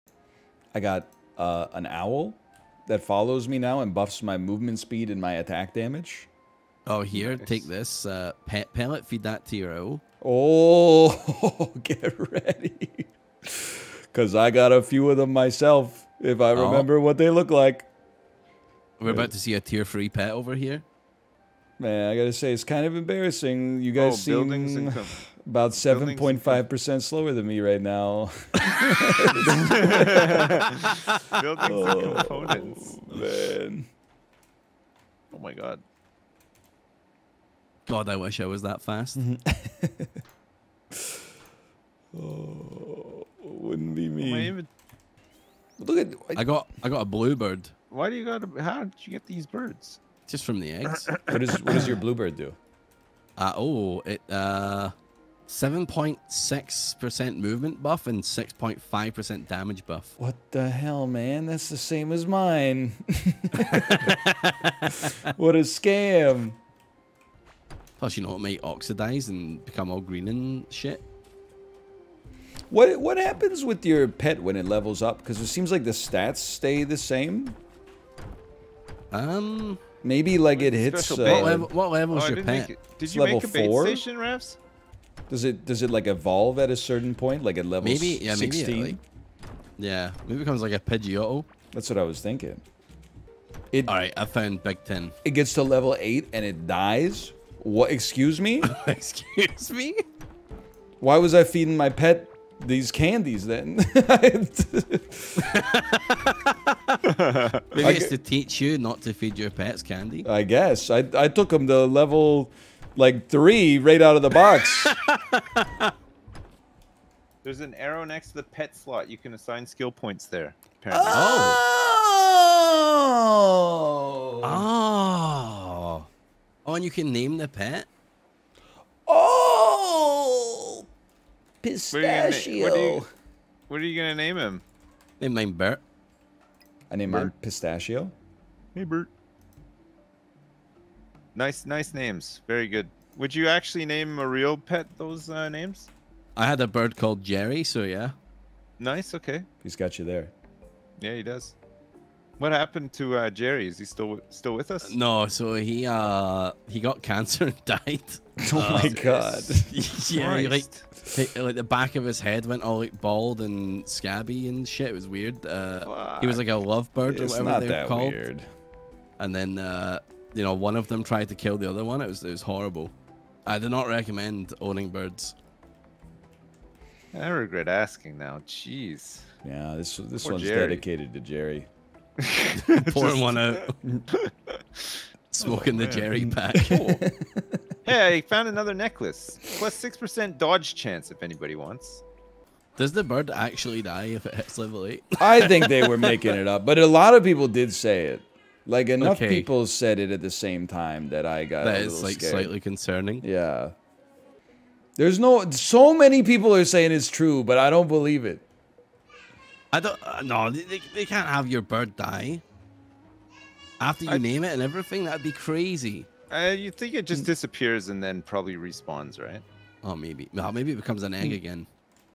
Chat gaslights Northernlion into thinking he is killing his own pet
this one goes out to jerry